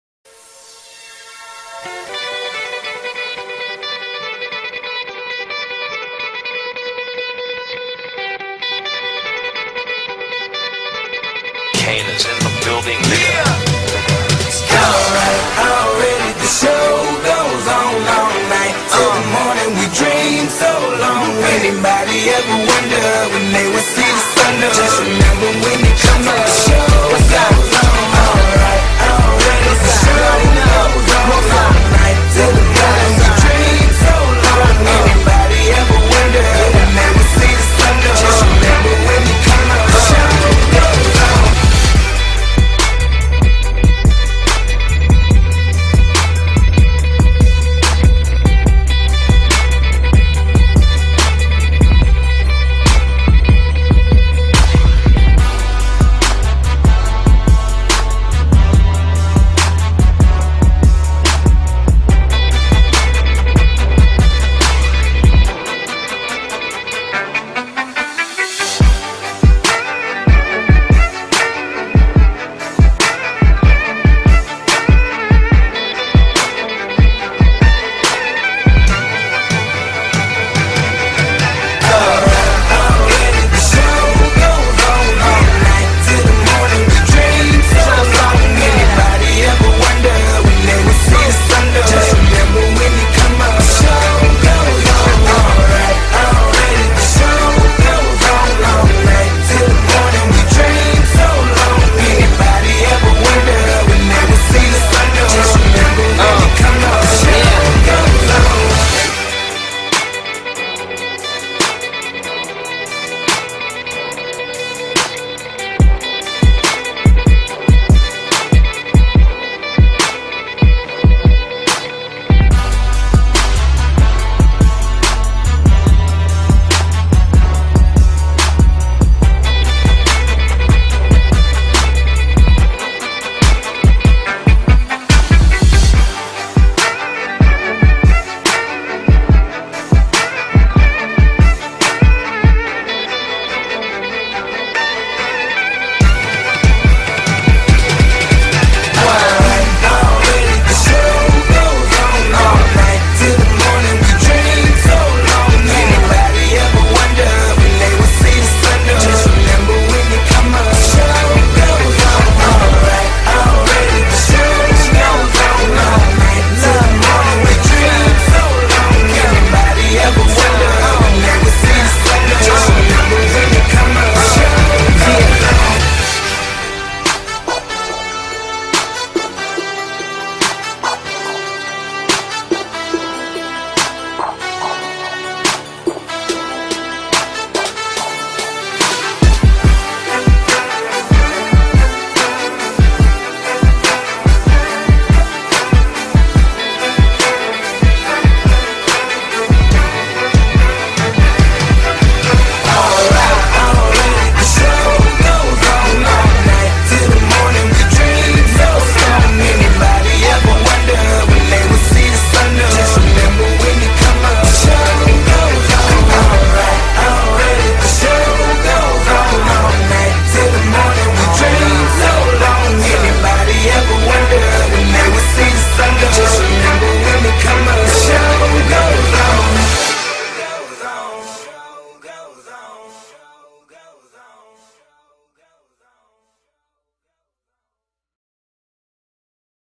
(HIN) HOT IMPORT NIGHTS 2016 - SAN JOSE, CA.
6-18-16  Whole lot of eye candy at the HIN event held at the Santa Clara Fairgrounds.